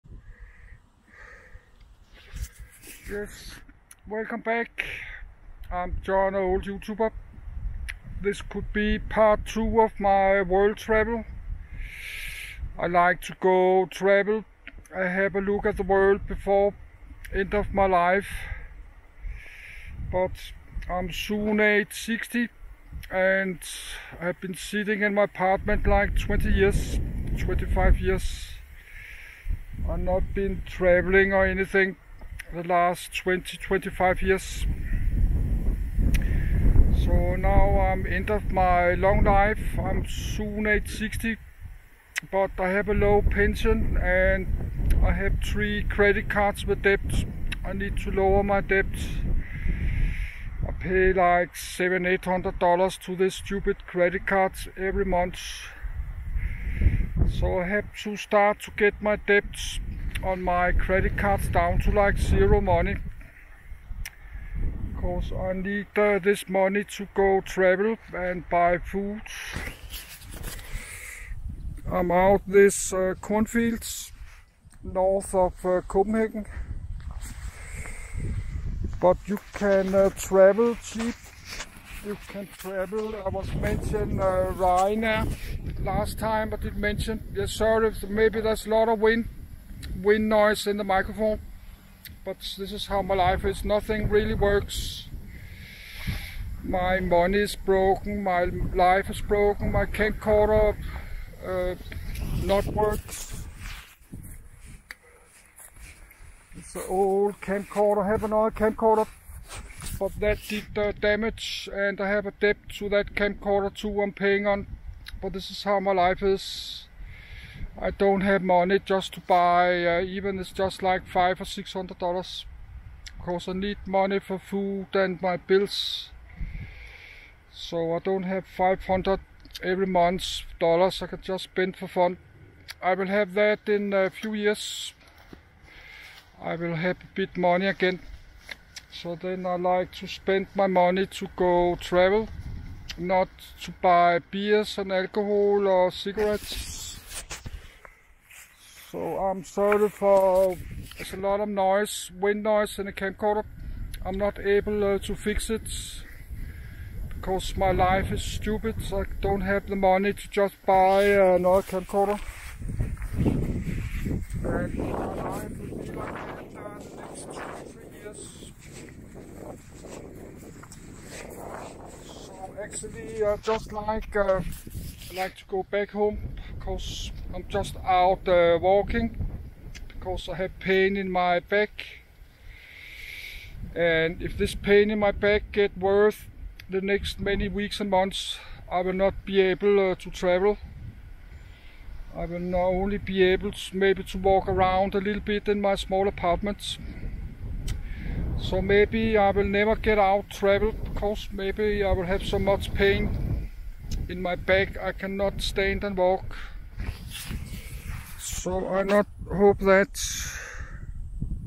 MongoTV_20000 - WORLD TRAVEL - Part 2 - Denmark - I Need Try Lower My Credit Card Debt To 0 Money
PART 2 - I need try lower my credit card debt, to like 0 money, the next 2 years. I would need that money every month, for my travel expenses. I think about, to buy a scooter 30, that drive long on liter fuel, and i don't need a driver likens for that too, in Denmark ?